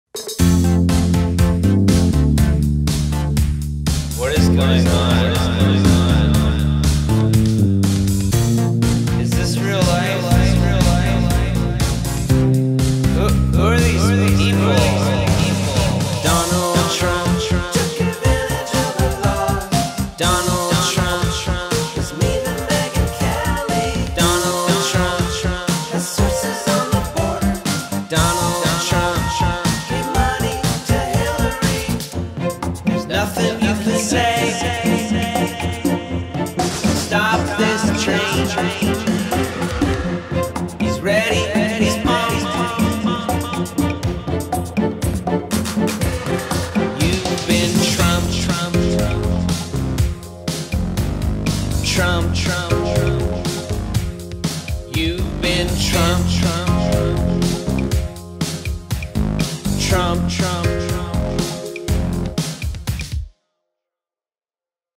Palmer Luckey Gets Trumped
Is this real life?
Who are these people?
Oh. God. No.

Donald Trump took advantage of the laws
Donald Trump was mean to Megan Kelley
Donald Trump has sources on the border
Donald Trump gave money to Hilary

Nothing you can say
Can stop this train
He’s ready, he’s pumped
You’ve been trumped